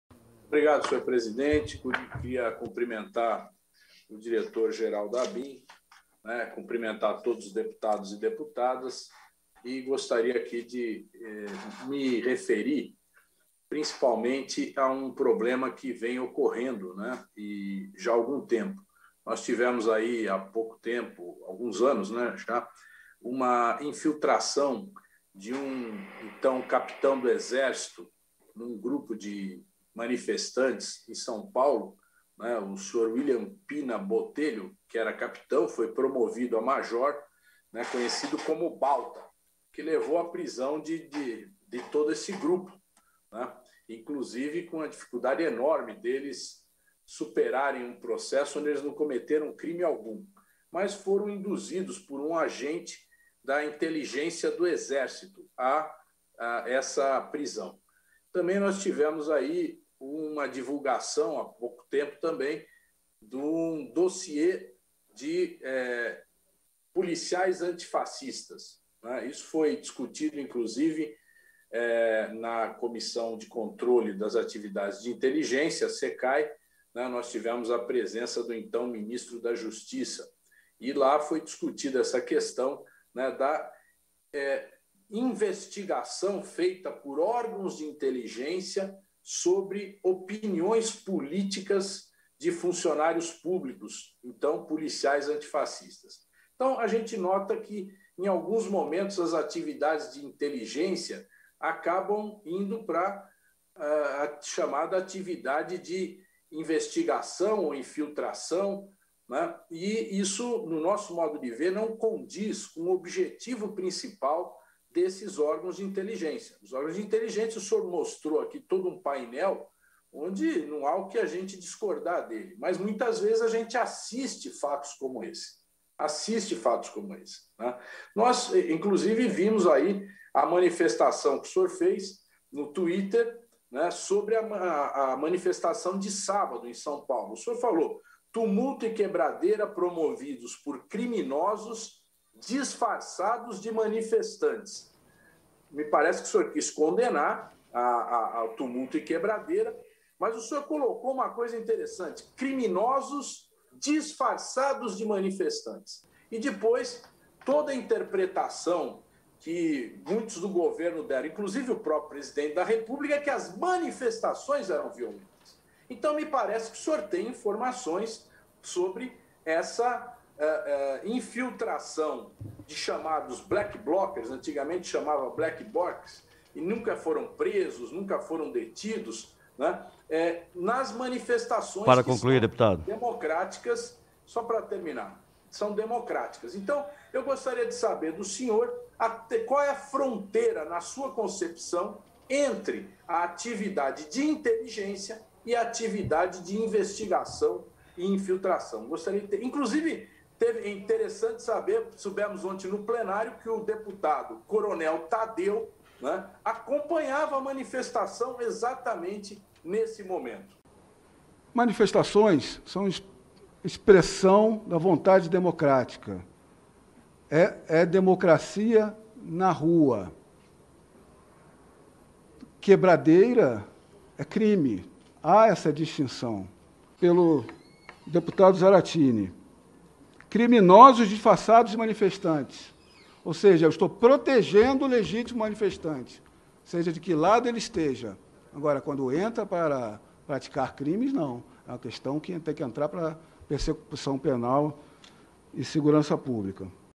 Presidente Bolsonaro é desmentido por diretor da Abin.
Diretor da Abin reiterou a diferença entre os manifestantes e os que tumultuaram a manifestação no últimos 3 de julho. A recepção foi feita em audiência pública na Comissão de Relações Exteriores e de Defesa Nacional da Câmara

Caiu por terra essa tentativa de Bolsonaro de imputar à esquerda a violência ocorrida.